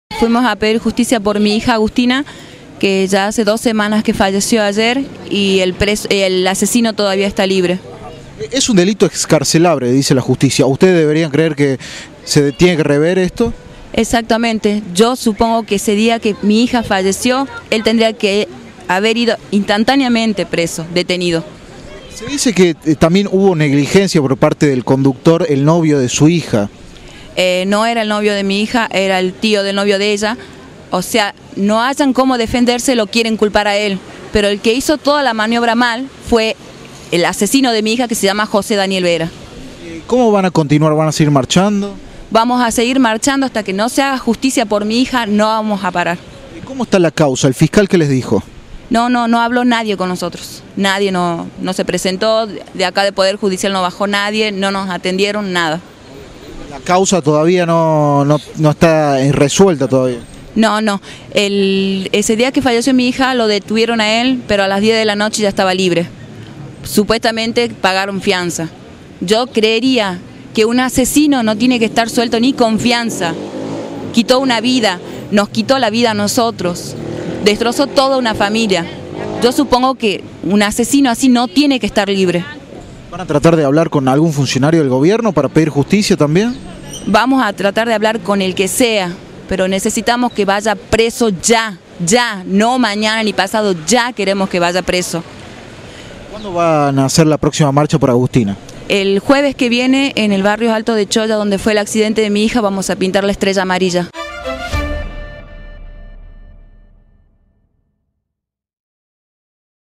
Nueva marcha en reclamo de justicia
PATRICIA PONCE / Madre de Agustina